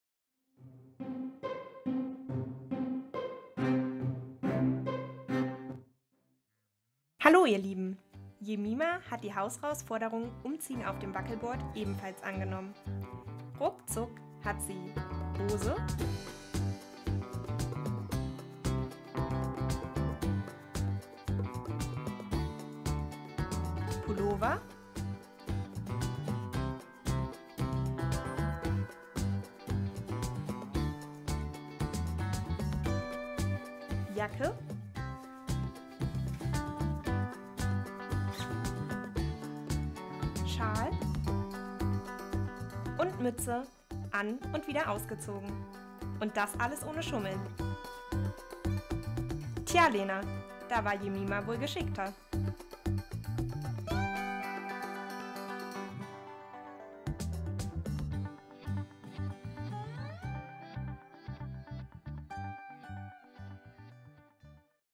Hau's raus-Forderung angenommen: umziehen auf dem Wackelbord von und mit Jemima
Jemima nimmt die hau's raus-Forderung an und zieht sich auf dem Wackelbord um. Schafft sie es schneller und besser als Lena?

Ihr findet uns übrigens auch auf Instagram @hausderjugendmarburg  @hausderjugendmarburg.familien